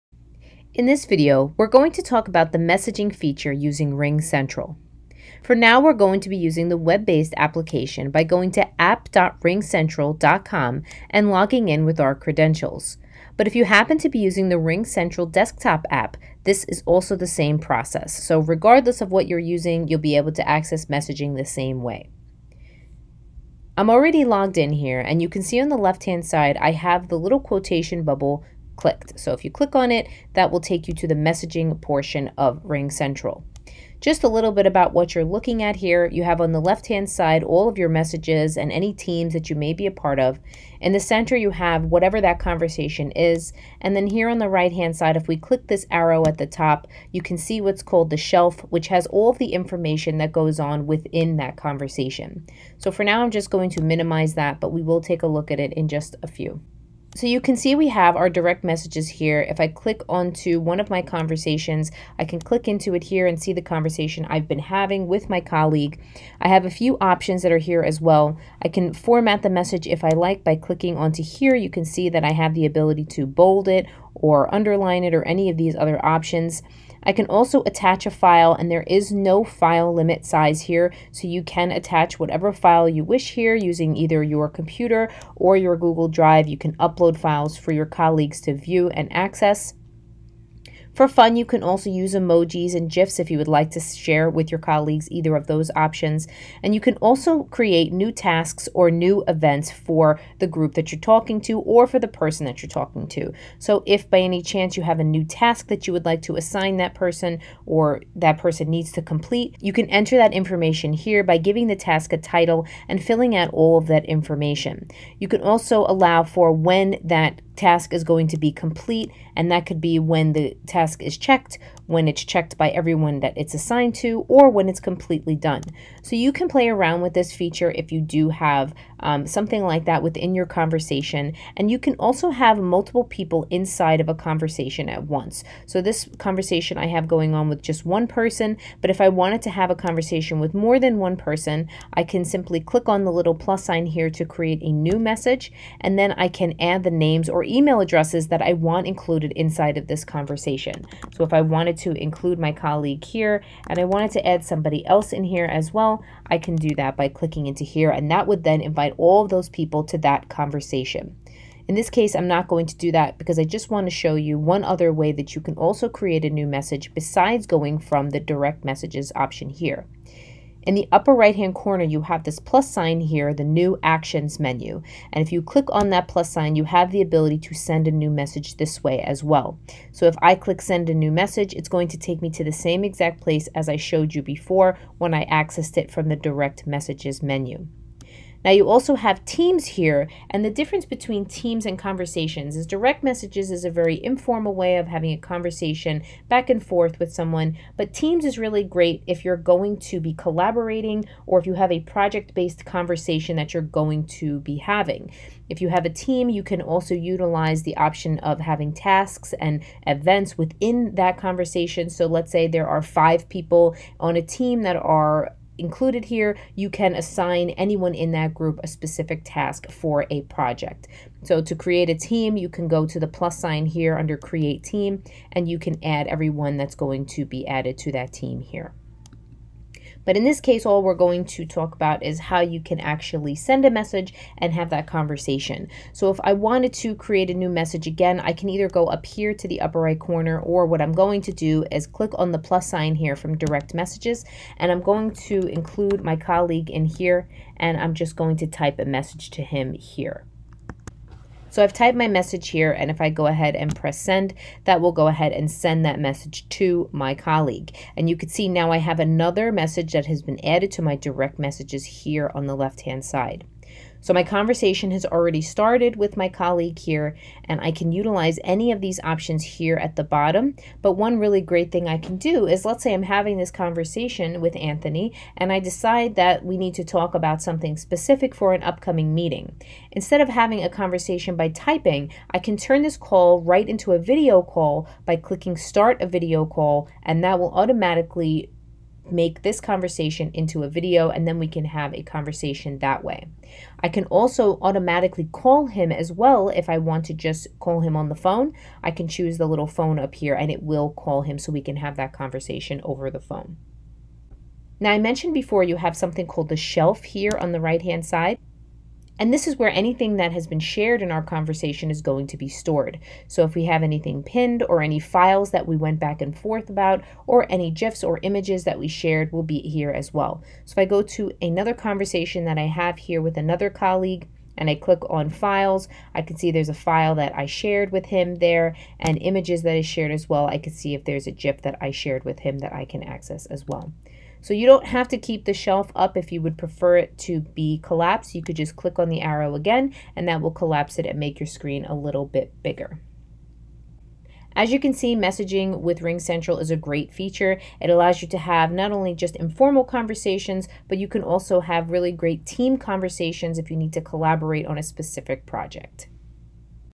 Messaging Using RingCentral
This video will walk you through RingCentral's messaging feature.